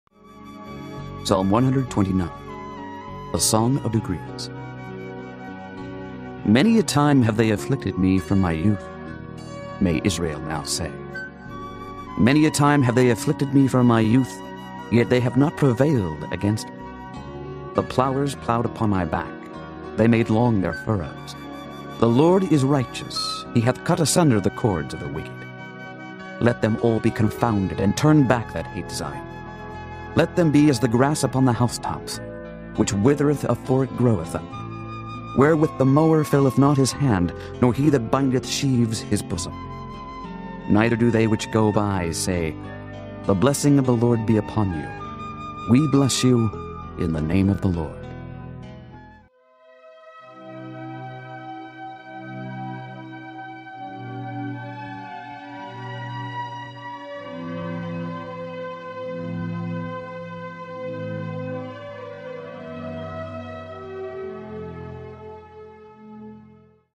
Psalm 129 Reading: Song of Victory over Zion’s Enemies (With words - KJV)
This video features a peaceful recitation of Psalm 129. The psalm reflects on the many struggles the Israelites faced throughout their history, yet their faith in God remained steadfast. The psalmist acknowledges the relentless persecution they endured, but ultimately declares their trust in God's righteous judgment and enduring love.

Find comfort and strength in this reading, especially if you are facing challenges in your own life.  Remember, God is faithful and will see you through difficult times.